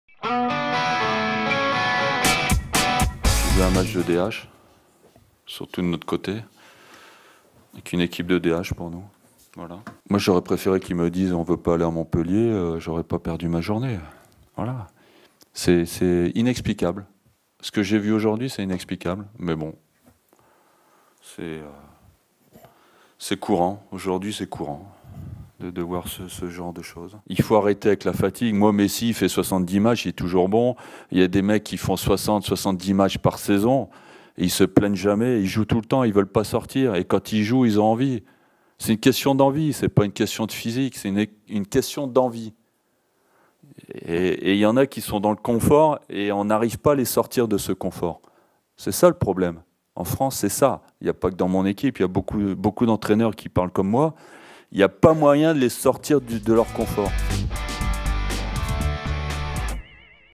F. Gillot : "J'ai vu une équipe de DH"
Francis Gillot l'entraîneur des Girondins de Bordeaux était très remonté contre ses joueurs à l'issue de l'élimination du club au scapulaire à Montpellier en Coupe de la Ligue (1-0)